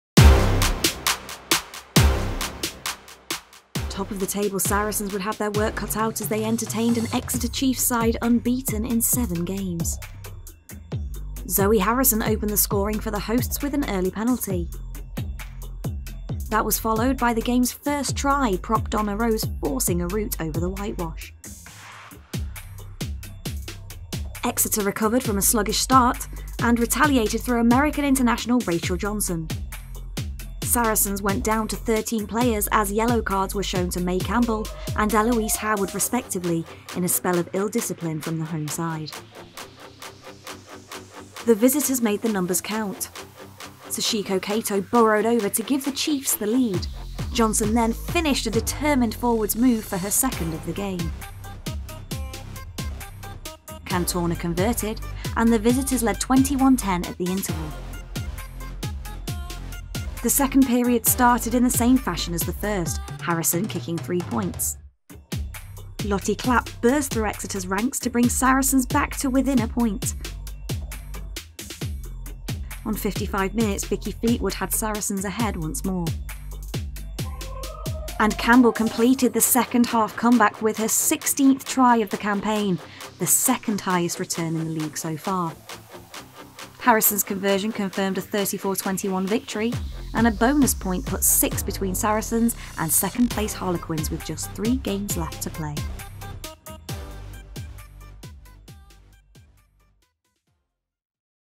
Saracens fight back to stun Exeter Chiefs | Round 15 highlights.
Extended highlights from the Allianz Premier 15s Round 15 fixture between Saracens and Exeter Chiefs at the StoneX Stadium.